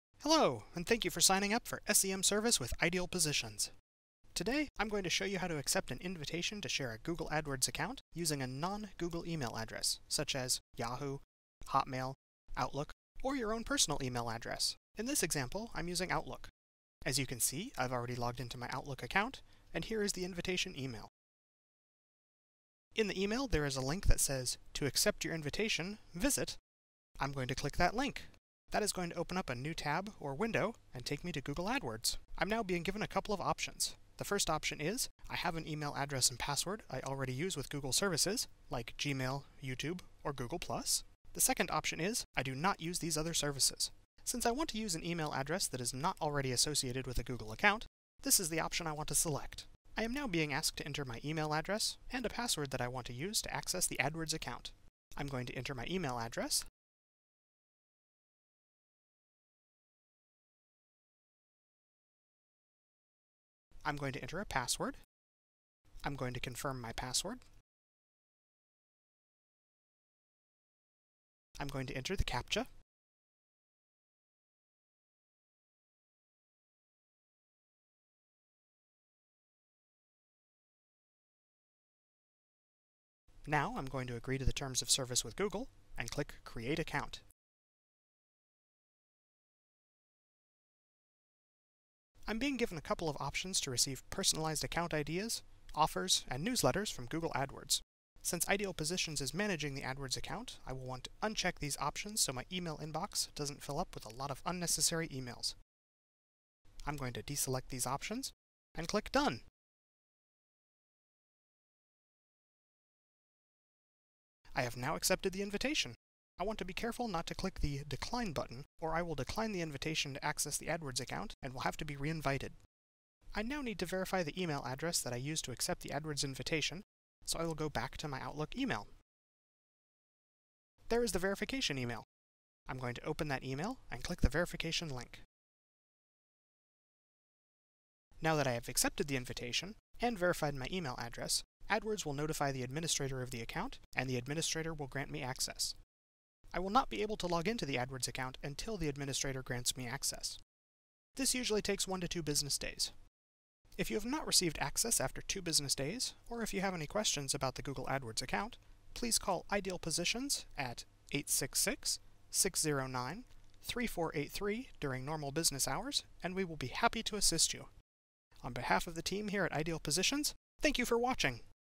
Accepting a Google AdWords Invite for Non-Gmail Users
The Google AdWords platform has a great invitation-based system designed to assist advertisers and agencies in sharing reporting access, account management responsibilities, and more. This system has its drawbacks however, most notably in the steps that need to be followed in order to accept an invitation. This video should help you accept an invitation to share access to a Google AdWords account, using a new Google account. If you already have a Google account (such as Gmail or Google Calendar), check Ideal Positions' YouTube Channel for an alternative instructional video aimed at users that already have such an account.